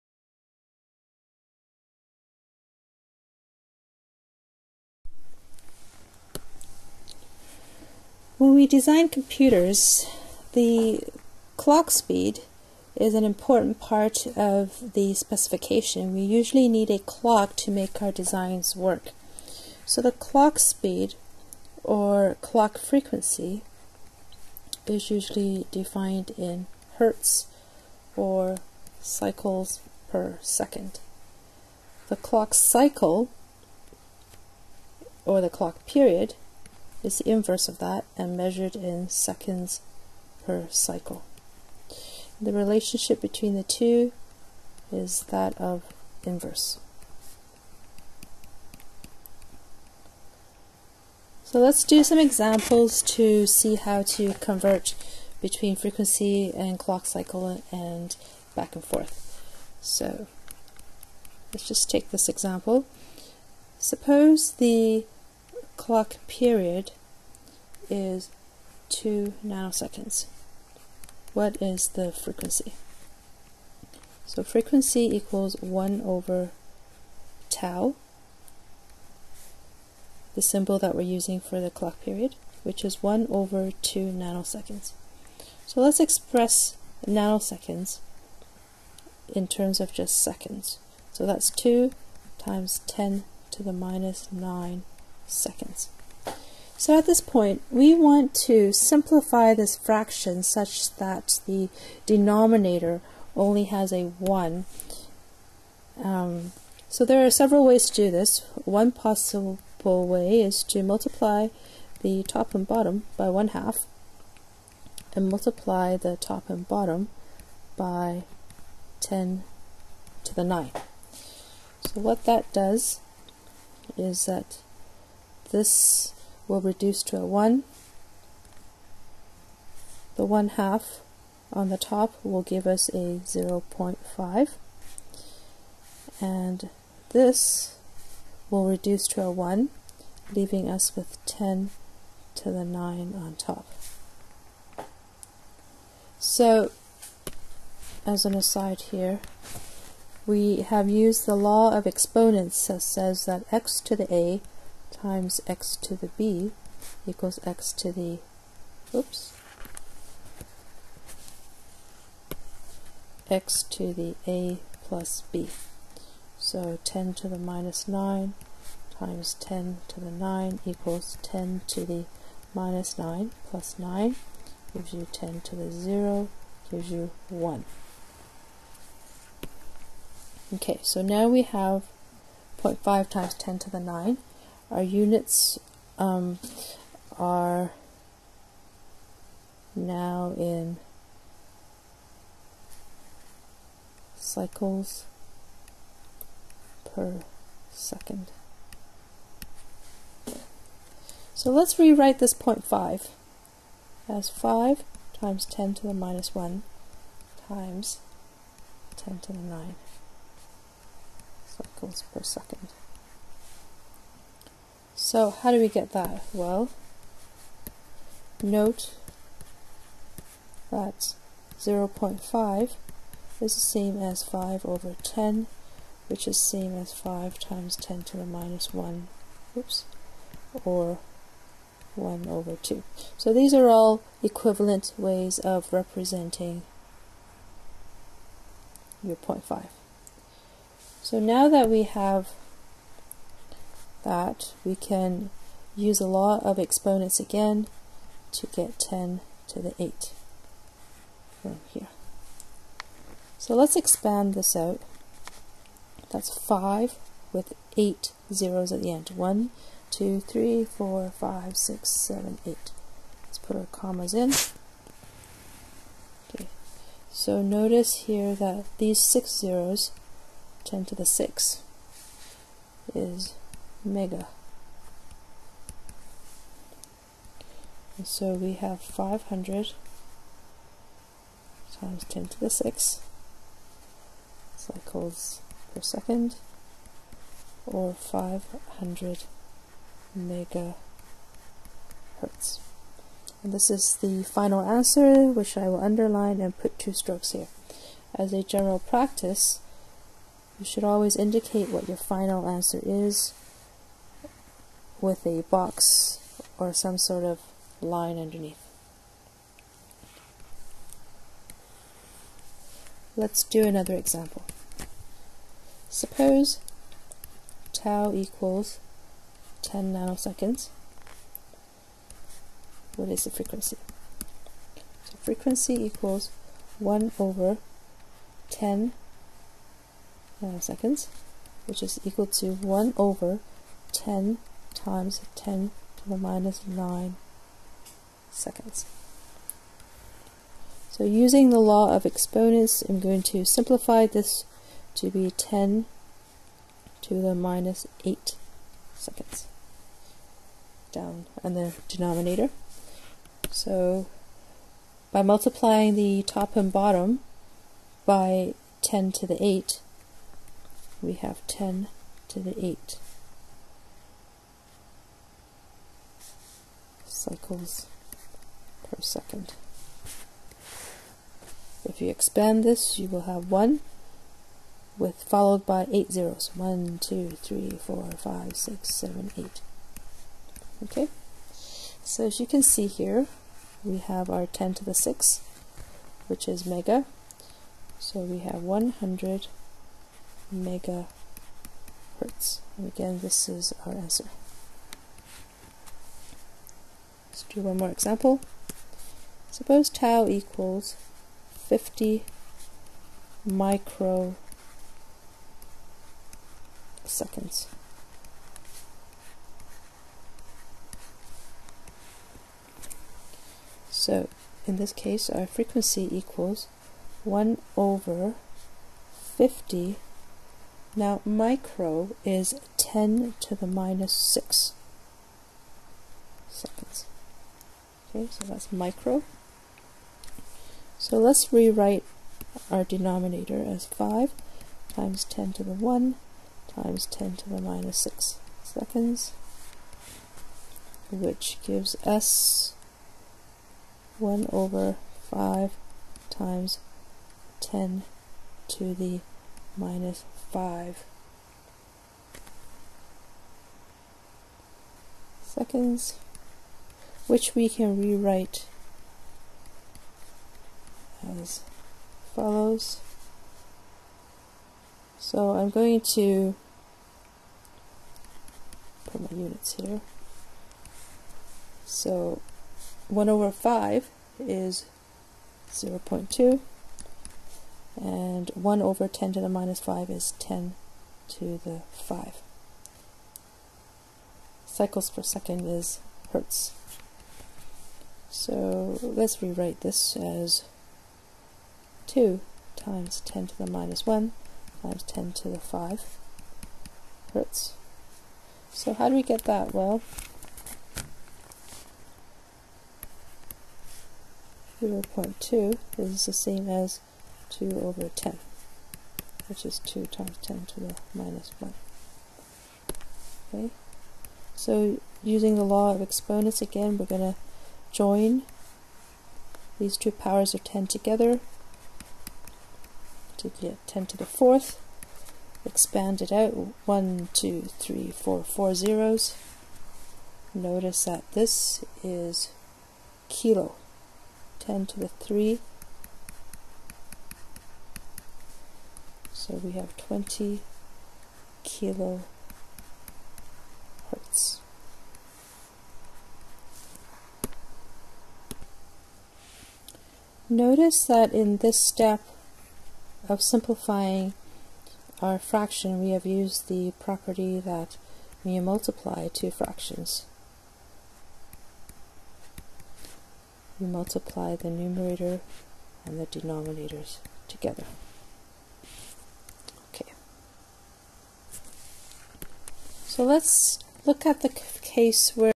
Tutorial 1 - Convert Period to Frequency
Tutorials for COMP2721
This is a step-by-step walk through.

Clock frequency (or clock speed) determines how fast the digital logic is being clocked in.
It also determines the clock period (or clock cycle time), ie, how much time is between two consecutive rising (or falling) edges of the clock.